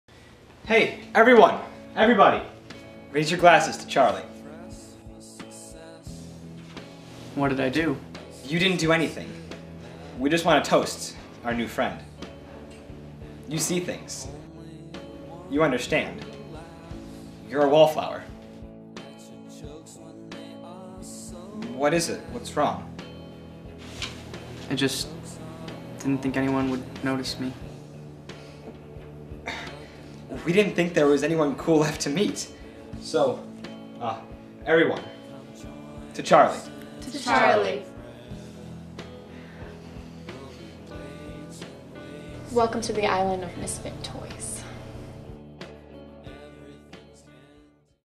A Toast To Charlie Cut Away - Lucky Seven Productions
A remake (cut away) of a scene from the movie "Perks of being a Wallflower". This video was made for a broadcasting class in high school in collaboration with all of the members of Lucky Seven Productions.